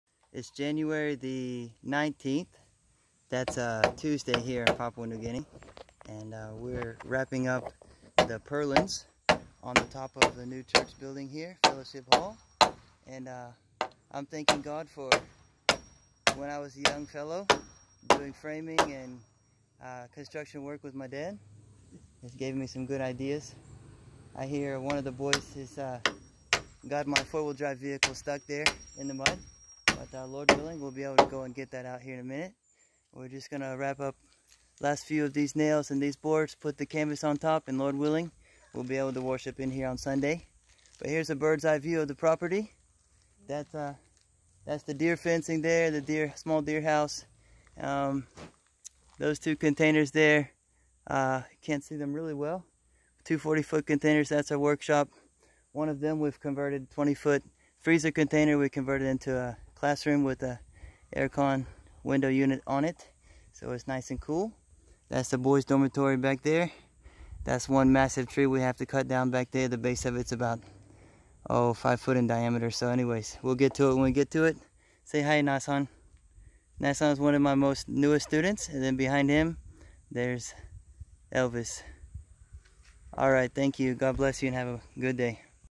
New church building progress
Short clip on the progress of our new church building in Papua New Guinea.